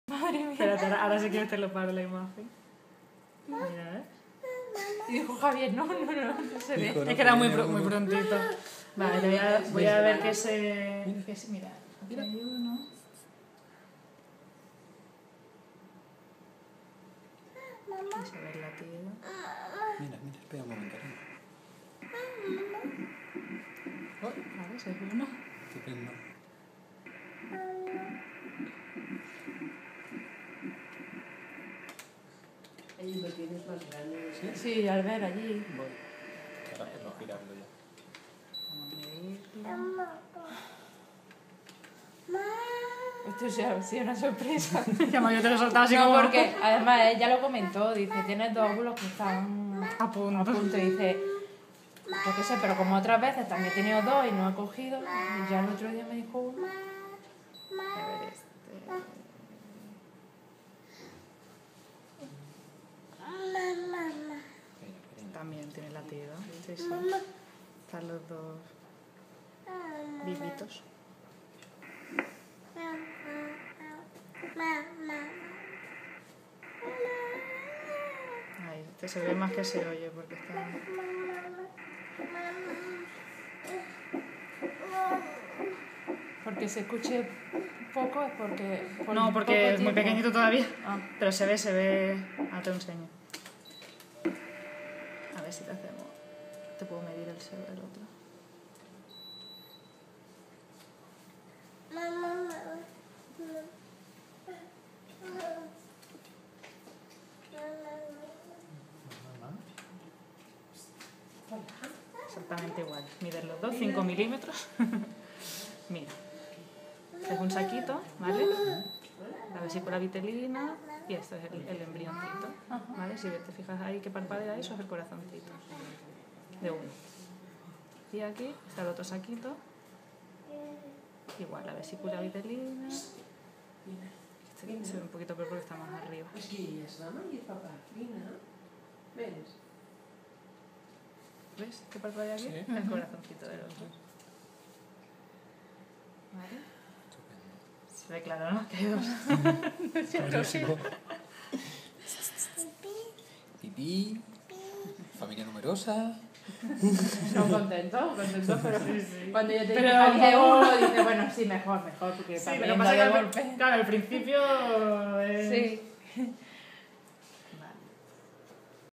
13-7-2012 (6+4) CONFIRMACIÓN MELLIZOS
Bromeábamos con la posibilidad de que fuesen Pili y Mili.
Ten cuidado con lo que deseas!

Seremos familia numerosa parece.